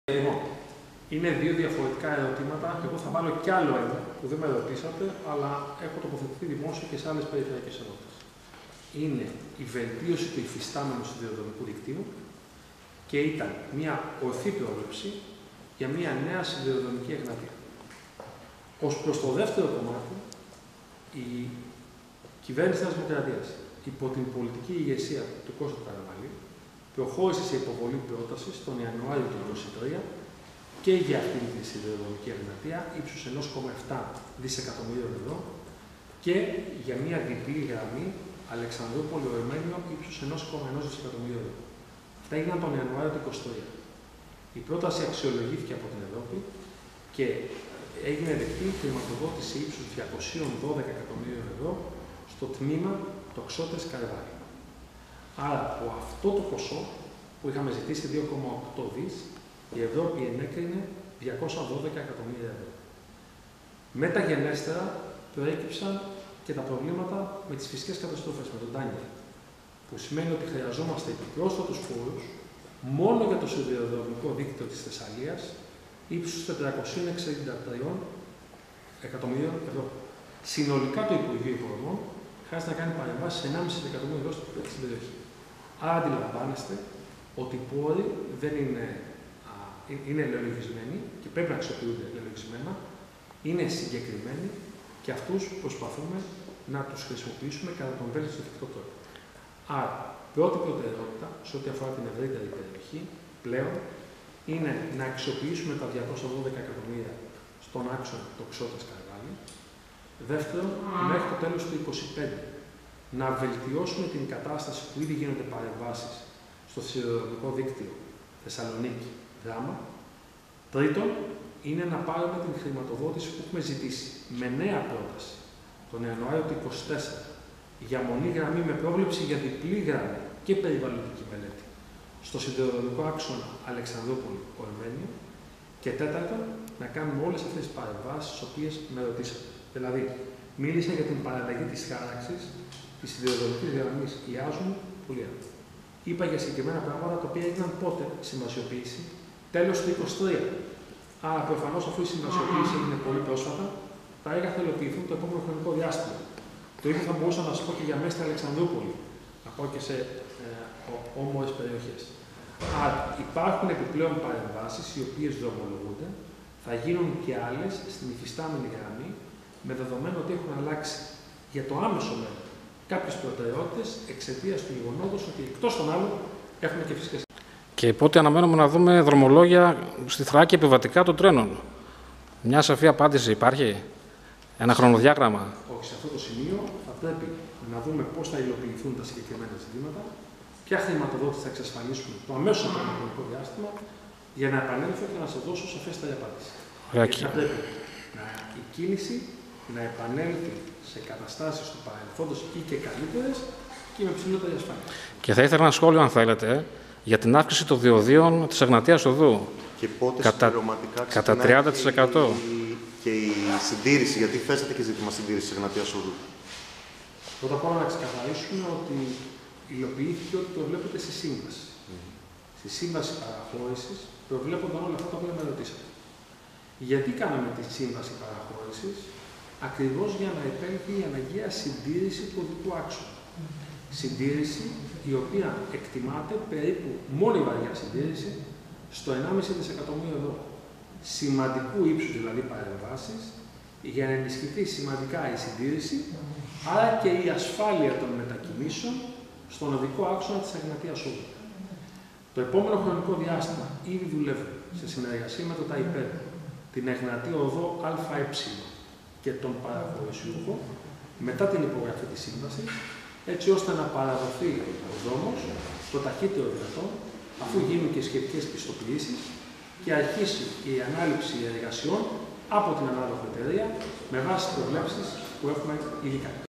Άγνωστο πότε θα δούμε ξανά δρομολόγια επιβατικών τρένων στη Θράκη
«Όχι, δεν μπορώ να σας δώσω σαφή απάντηση», είπε ο Υπουργός Υποδομών και Μεταφορών σε σχετική ερώτηση της «Φωνής της Ροδόπης» - «Η αύξηση στα διόδια της Εγνατίας Οδού προβλεπόταν από τη σύμβαση παραχώρησης»